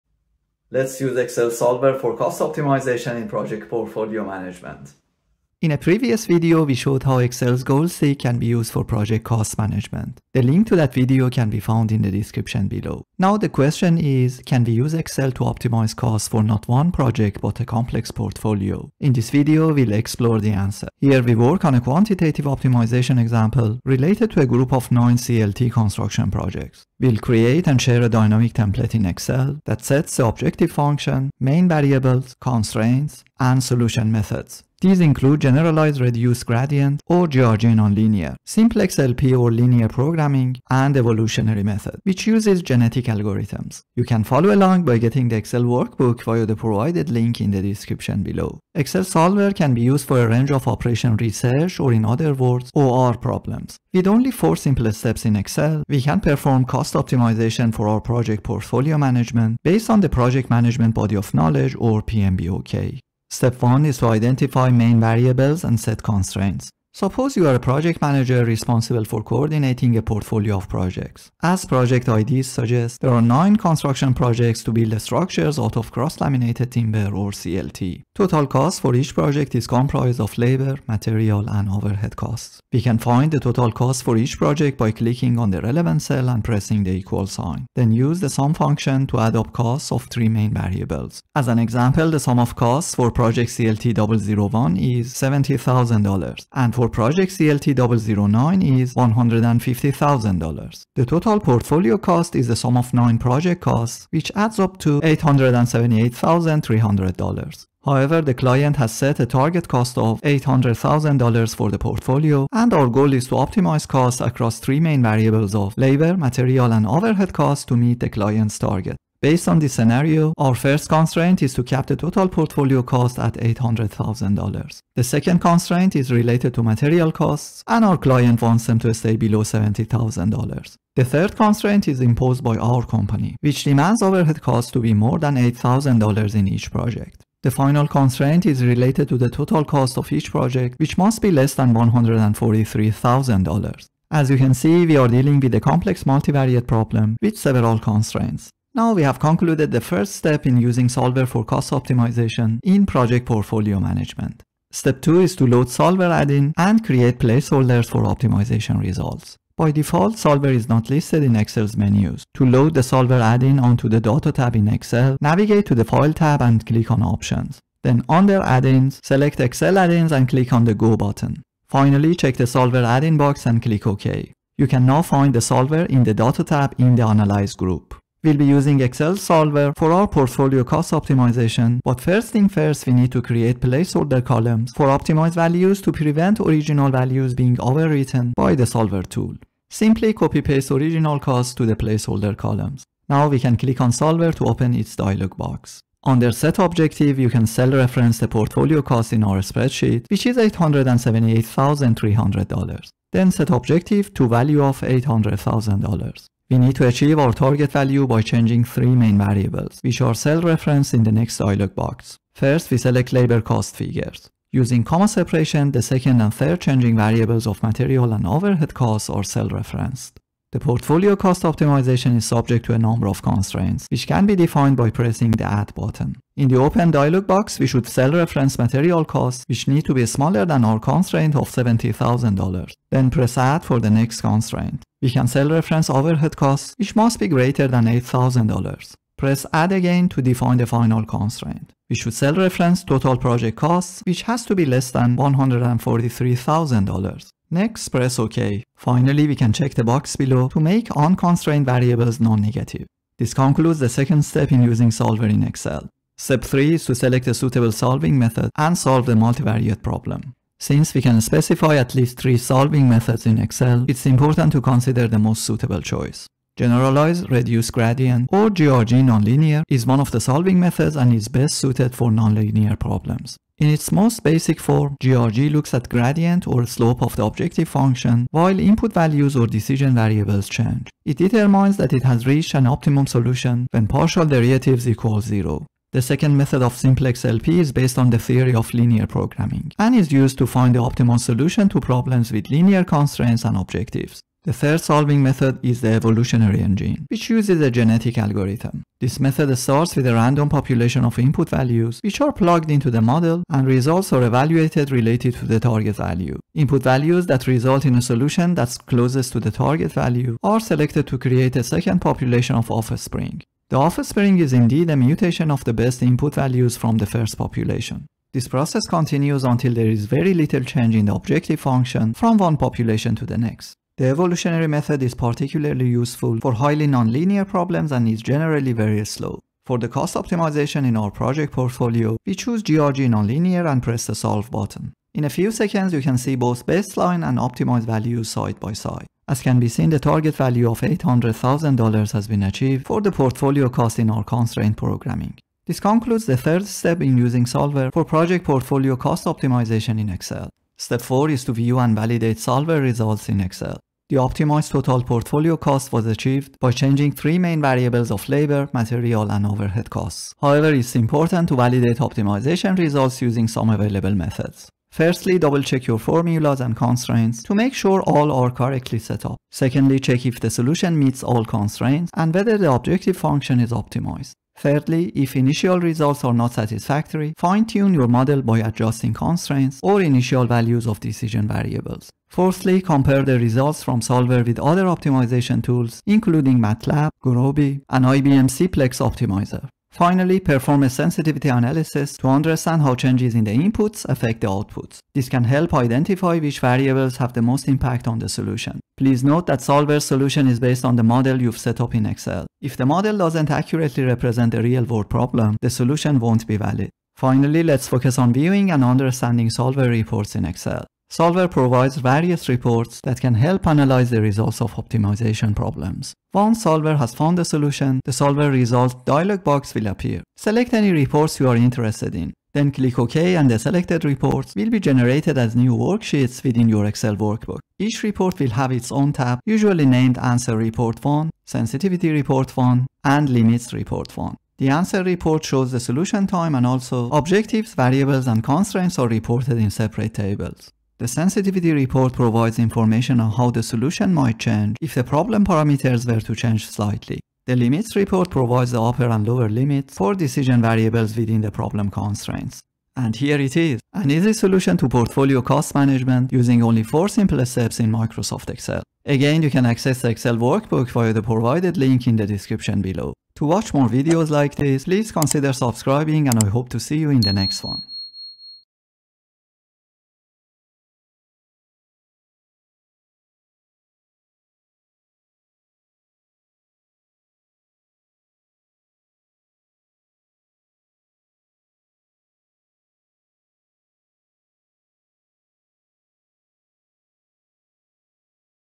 Optimizing Project Costs ❓ EXCEL SOLVER with 4 Easy Steps - Example of Project Portfolio Management
In this video of engineeringmanagementacademy ExcelSolver is used for projectportfolio by drmehrdadarashpour
 ⌛ TIMESTAMPS
0:00 - Introduction to Excel Solver & Operations Research in Excel
0:26 - Excel’s Dynamic Template, Solver Parameters, GRG Nonlinear, Simplex LP, Evolutionary Method, Genetic Algorithm
1:02 - Step 1 (Identify Main Variables (Multivariate) & Set Constraints)
2:34 - Step 2 (Load Solver Add-in & create Placeholders for Optimization Results)
4:16 - Step 3 (Select a Suitable Solving Method & Solve the Multivariate Problem)
5:50 - Step 4 (View & Validate Solver Results in Excel)
6:50 - Generate & Understand Solver Reports in Excel - Answer, Sensitivity Analysis, Limits

In a previous video, we showed how Excel’s Goal Seek can be used for project cost management. The link to that video can be found in the description below↓ Now the question is: Can we use Excel to optimize costs for not one project but a complex portfolio? In this video, we’ll explore the answer. Excel Solver can be used for a range of Operations Research or, in other words, OR problems. Here, we work on a quantitative optimization example related to a group of 9 CLT construction projects. We’ll create & share a dynamic Template in Excel that sets the Objective Function, Main Variables, Constraints & solution methods.
With only 4 simple steps in Microsoft Excel, we can perform cost optimization for our project portfolio management based on the project management body of knowledge or PMBOK.
Step 1 is to Identify Main Variables (Multivariate) & Set Constraints
Suppose you are a project manager responsible for coordinating a portfolio of projects. As project IDs suggest, there are 9 construction projects to build structures out of cross-laminated timber or CLT. Total cost for each project is comprised of labor, material & overhead costs. Our first constraint is to cap total portfolio cost at $800,000. Second constraint is related to material costs & our client wants them to stay below $70,000. The 3rd constraint is imposed by our company, which demands overhead costs be more than $8,000 in each project. Final constraint is related to total cost of each project, which must be less than $143,000. As you can see, we are dealing with a complex multivariate problem.
Step 2 is to Load Solver Add-in & create Placeholders for Optimization Results
By default, Solver is not listed in Excel’s menus. To load Solver add-in onto Data tab in Excel, navigate to ‘File’ tab & click on ‘Options’.
Now, we can Click on Solver to open its dialog box. Under ‘set objective’, you can cell reference portfolio cost in our spreadsheet.
Step 3 is to Select a Suitable Solving Method & Solve the Multivariate Problem
Generalized Reduced Gradient or GRG Nonlinear is one of solving methods & is best suited for nonlinear problems. In its most basic form, GRG looks at gradient or slope of objective function while input values or decision variables change. It determines that it has reached an optimum solution when partial derivatives equal zero.
The 2nd method of Simplex LP is based on the theory of linear programming & is used to find the optimal solution to problems with linear constraints.
The 3rd solving method is the Evolutionary engine, which uses a Genetic Algorithm. This method starts with a random “population” of input values, which are plugged into the model & results are evaluated relative to the target value. Input values that result in a solution that’s closest to the target value are selected to create a second population of offspring. The offspring is indeed a “mutation” of the best input values from the first population. This process continues until there is very little change in the objective function from one population to the next. The evolutionary method is useful for nonlinear & is generally slow.
Step 4 is to View & Validate Solver Results in Excel
Double-check your formulas & constraints to make sure all are correctly set up. Check if the solution meets all constraints & whether the objective function is optimized. If initial results are not satisfactory, fine-tune your model by adjusting constraints or initial values of decision variables. Compare the results from Solver with other optimization tools, including MATLAB, Gurobi & IBM CPLEX Optimizer. Finally,  Perform a sensitivity analysis to understand how changes in the inputs affect the outputs.
Sensitivity Report provides information on how the solution might change if the problem parameters were to change slightly. The Limits Report provides the upper and lower limits for decision variables within the problem constraints.